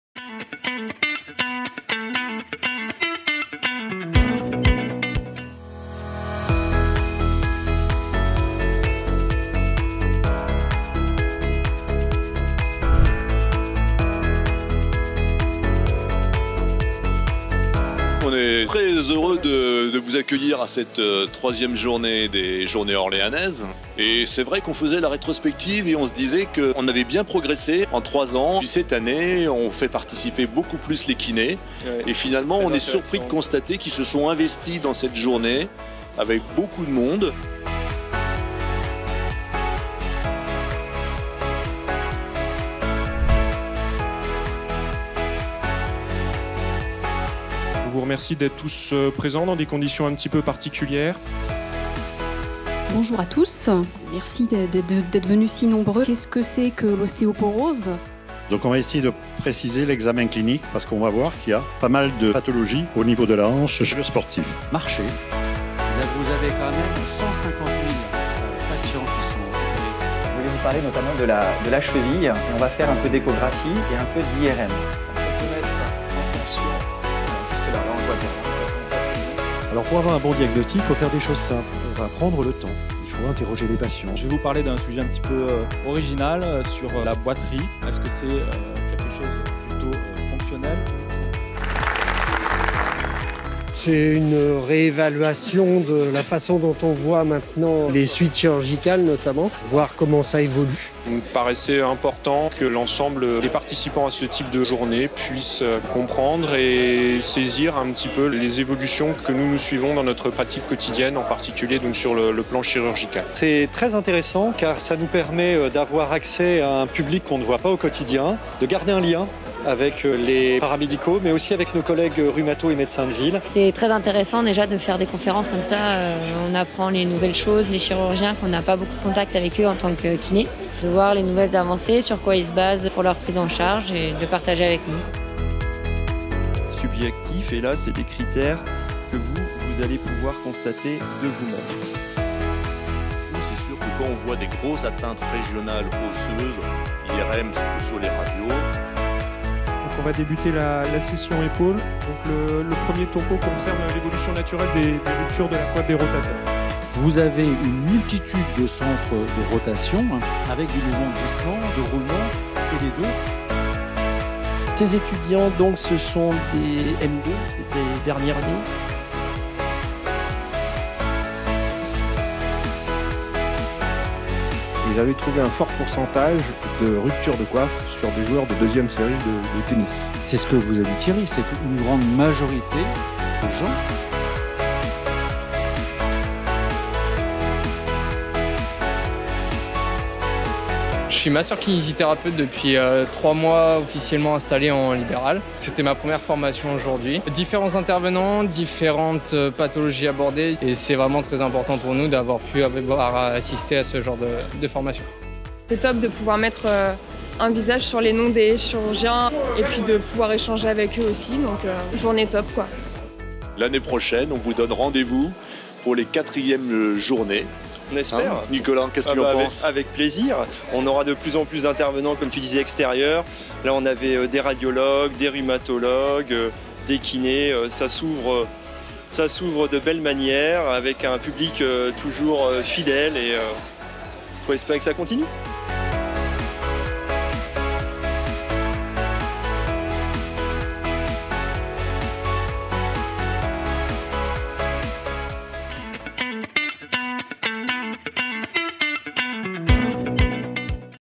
3ème Journée Médico Chirurgicale Oréliance - 10/10/2020
Revivez les moments forts de la 3ème Journée Médico Chirurgicale Oréliance du 10 octobre 2020 !

Cette journée était consacrée à “L’évolution des techniques médico-chirurgicales et de rééducation pour la prise en charge des pathologies neuro-musculosquelettiques chez le sportif”.
Les participants ont pu assister à différentes interventions d’experts, à des plateaux TV et ateliers pratiques ; le tout dans une atmosphère conviviale.